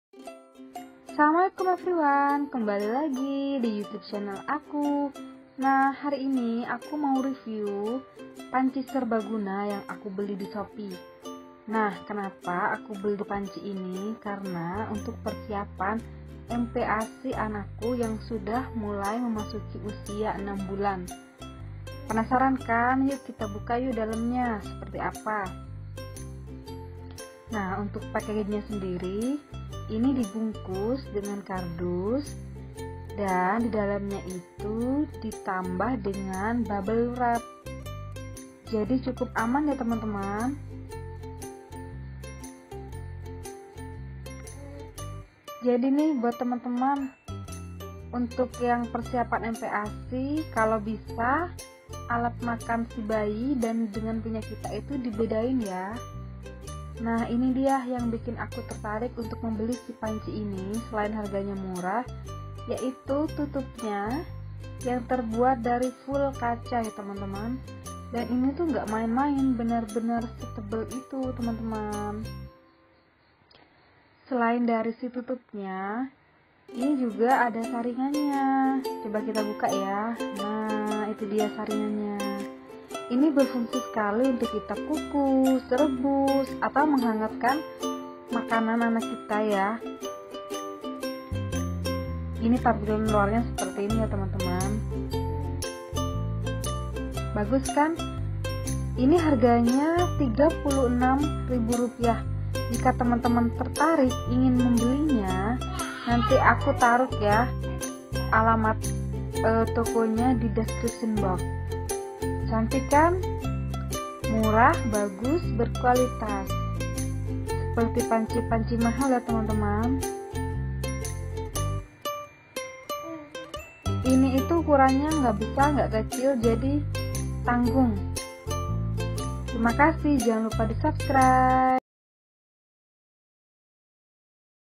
REVIEW ALAT MASAK MPASI TERMURAH DI SHOPEE...!!! WAJIB PUNYA /
Hai mom, siapa nih yg udh ga sabar nyipin mpasi untuk si kecil kesayangan kita...??? Nah aku kasi recomendasi buat teman2 alat masak murah yg aku beli di shopee..murah tapi bahannya bagus ya teman2. Kalau bisa alat masak kita harus di bedain ya, kasian kan kalau habis masak pedes2 trus kita menggunakan alat masak yg sama..nah...makanya simak video aku sampai habis.
oy ini dia apamat shopee nya
nailaadibah